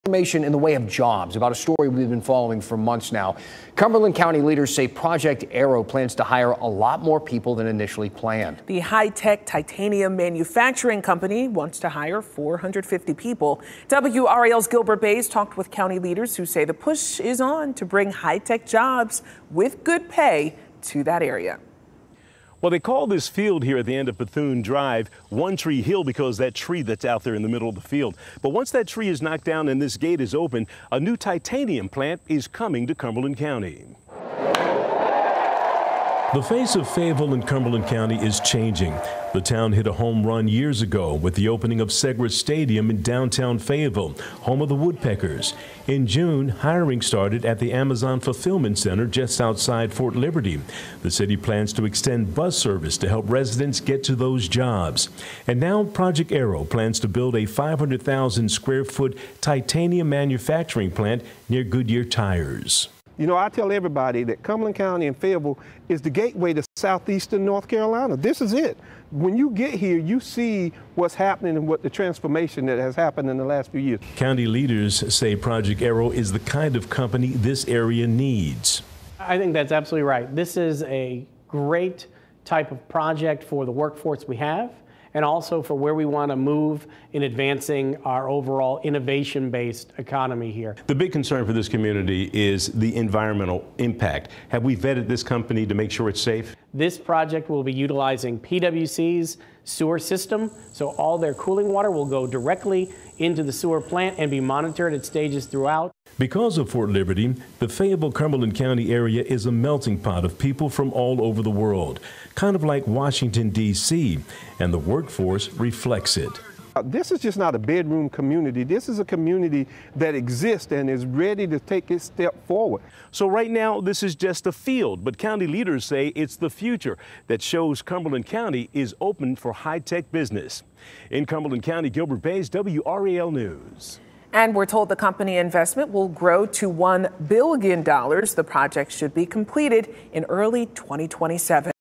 Fayetteville leaders explain vision for 'taking step forward' through Project Aero
Project Aero plans to build a 500,000-square-foot Titanium Manufacturing Plant near Goodyear Tires in Fayetteville.